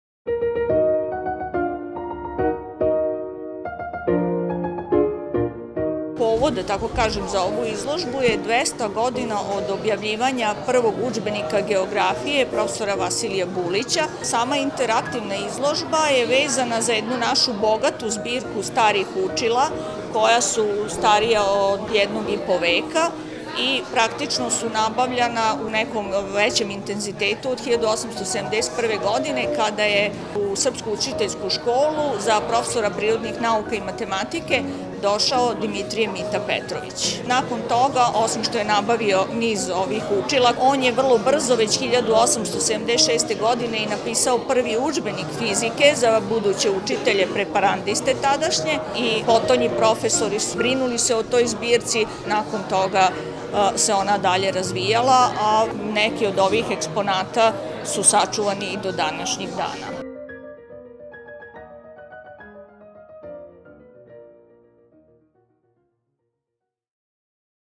Sombor je otvoren-Interaktivna izložba na Pedagoškom fakultetu
Na Pedagoškom fakultetu, u završnici programa povodom obeležavanja Dana grada, otvorena je interaktivna izložba pod nazivom „Dva veka prirodno-naučnog obrazovanja učitelja u Somboru“.
Organizatori su Pedagoški fakultet i UG „Norma“ Sombor, a ulaz je slobodan.